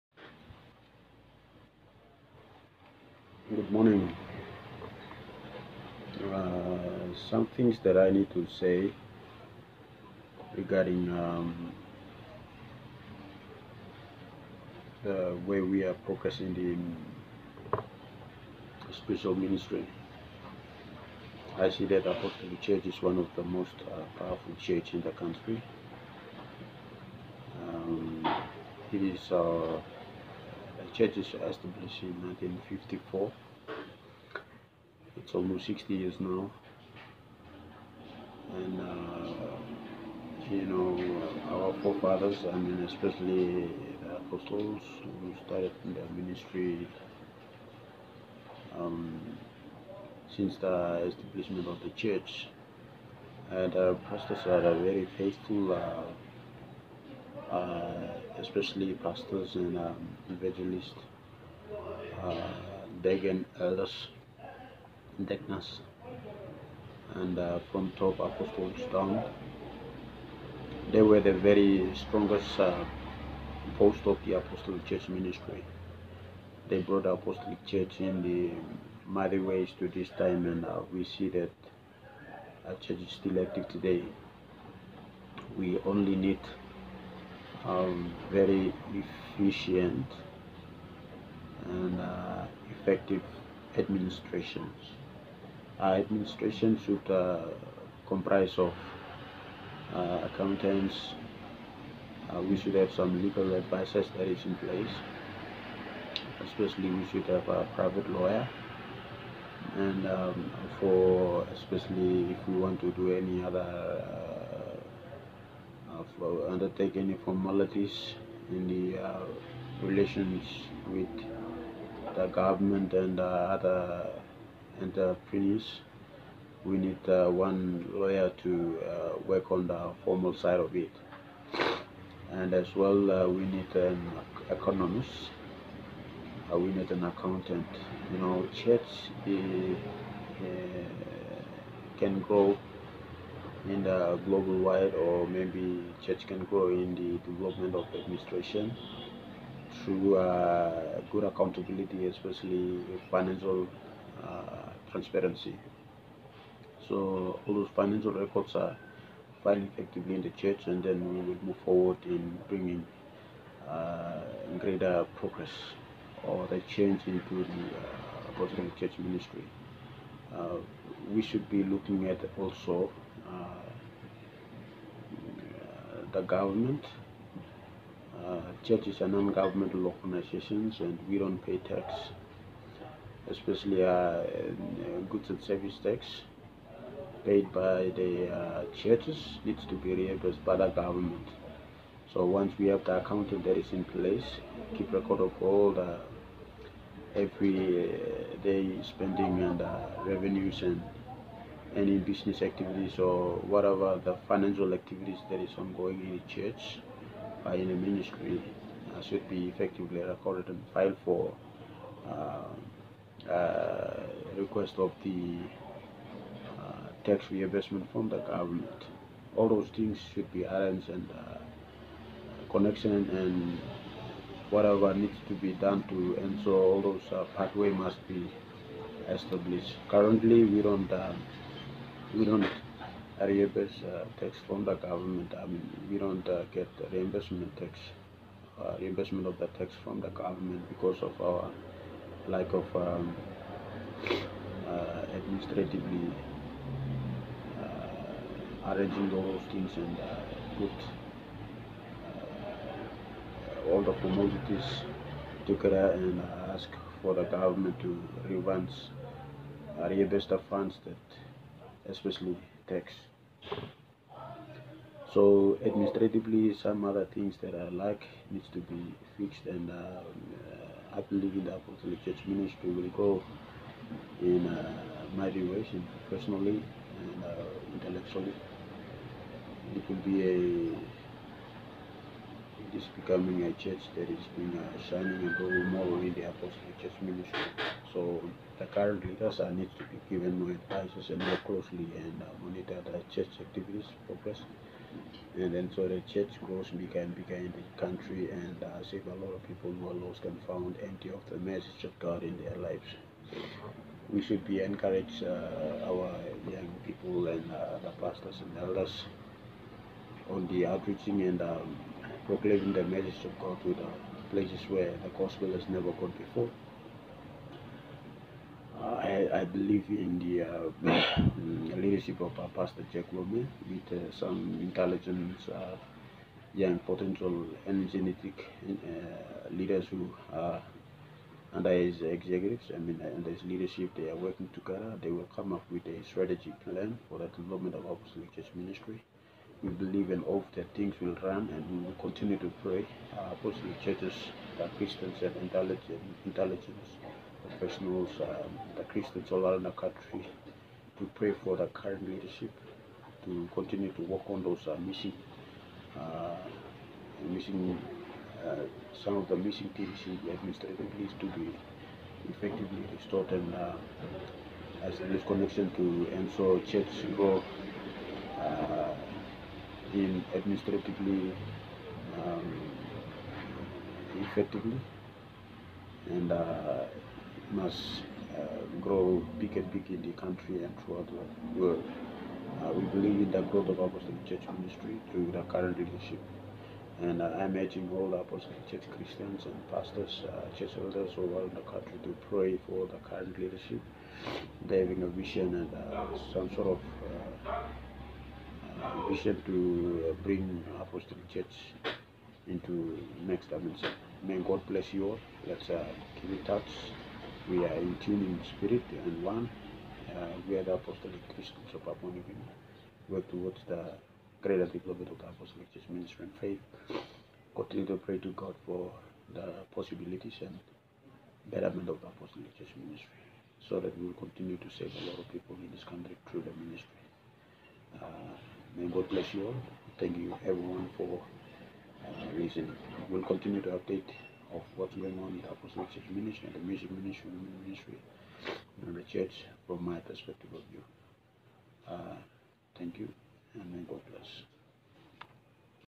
Apostolic Church of Papua New Guinea
Apostolic Church of Papua New Guinea is a spiritual ministry of God which established in 1954 in the country Papua New Guinea.  The church is now 63 years old.  It is still active today.  It is based on fivefold ministries. We have following ministries operating in this organisation :

1. Youth Ministry
2. Women Ministry
3. Ascension Ministry
5. Others

The Apostolic Church is one of the God's church and we believe in faith through prayers to God. It is a spiritual ministry that operates on the works of Holy Spirit of God.

The God's words are uttered through prophets base on needs of people's life.  The word of God is clear through the conviction of Holy Spirit.

The Apostolic Christians are praying for the revival of God. The God's power is awesome and we feel his presence through worships and praises to him.

Like other spiritual ministries this ministry will grow in the mighty ways and help rescue many souls that are perishing in the country.